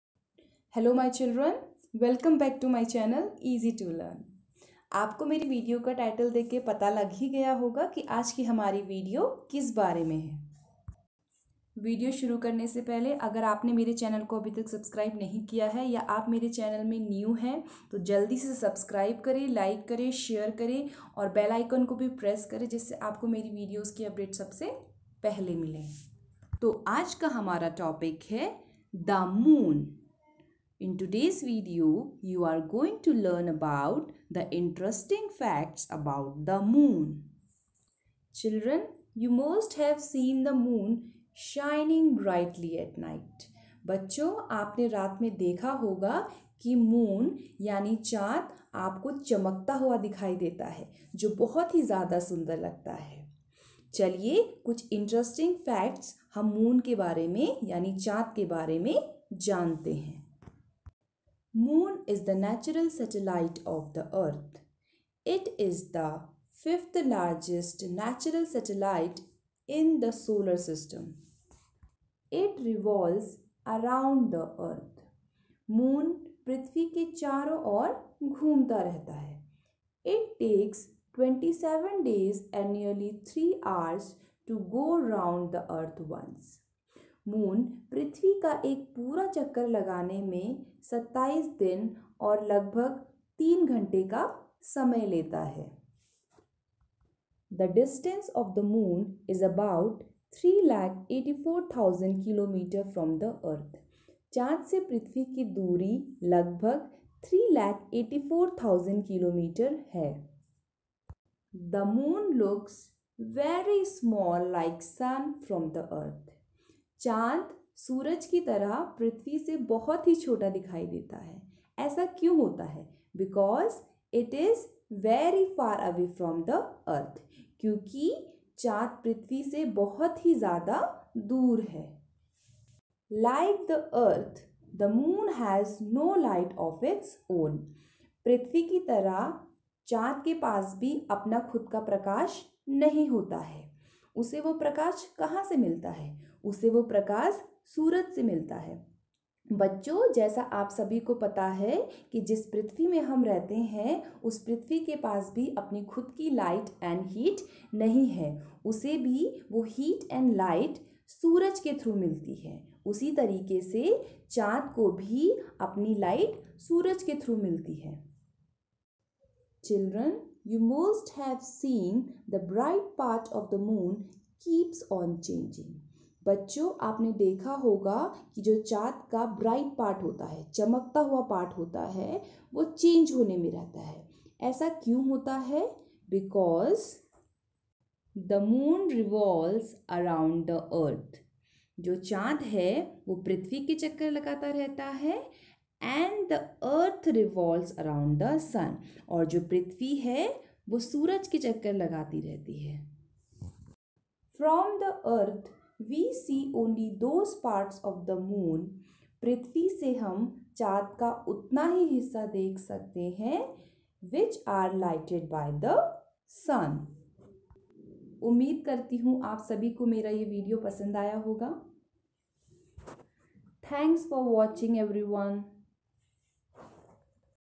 Interesting facts about the Moon# the Moon
In this video ,I have explained the interesting facts about the moon..